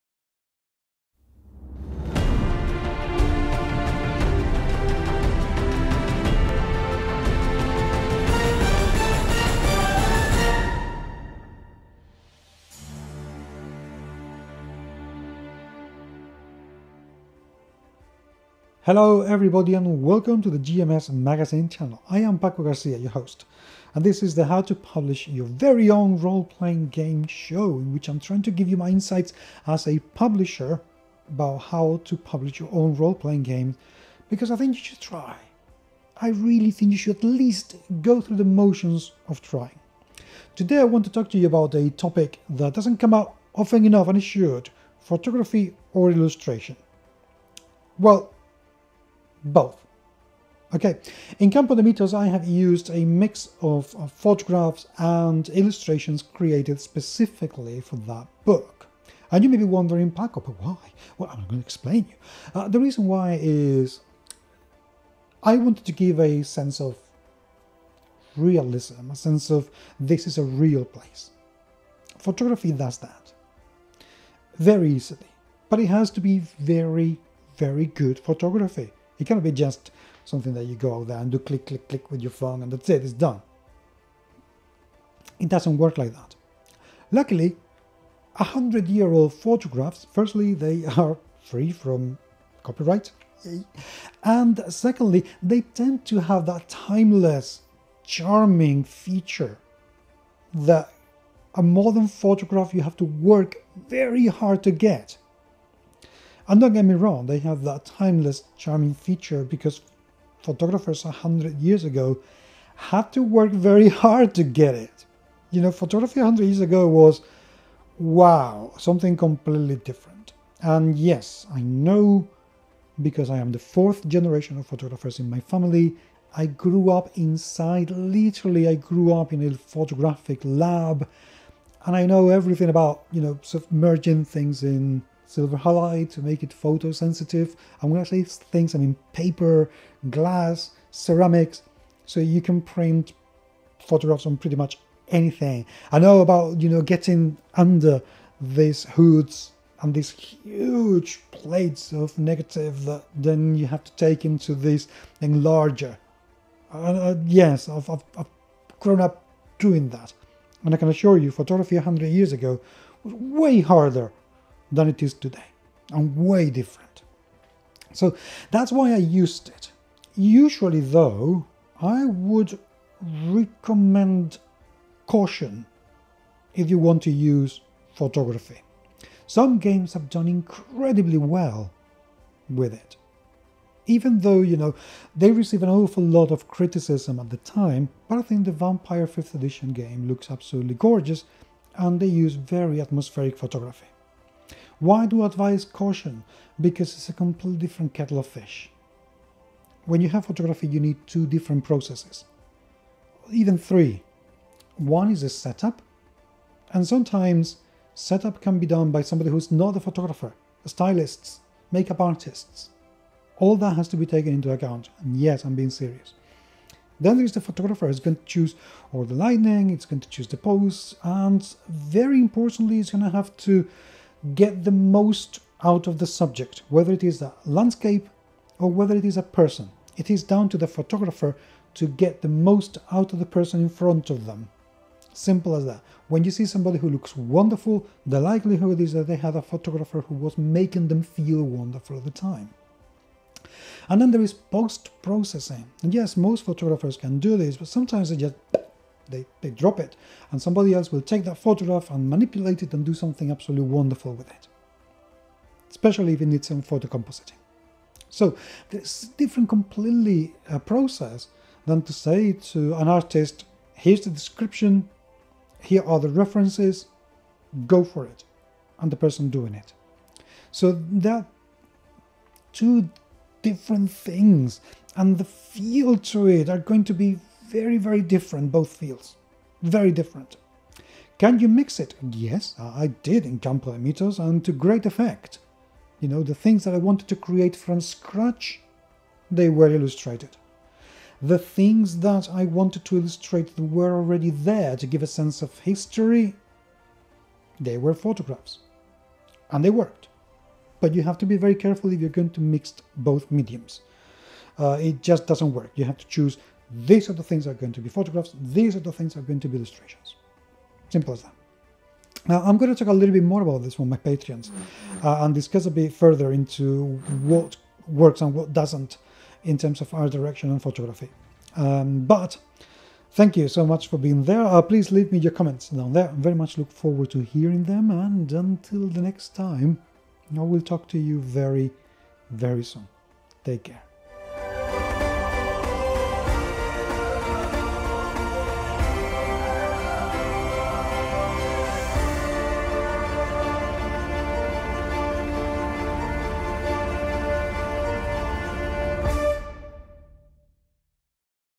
How to publish your RPG Ep. 26: Should you use Photography?
The photographic medium is closer to most people than illustrations. Even though mastering photography itself is not easy, getting pleasant results is relatively easy.

So why not use it more?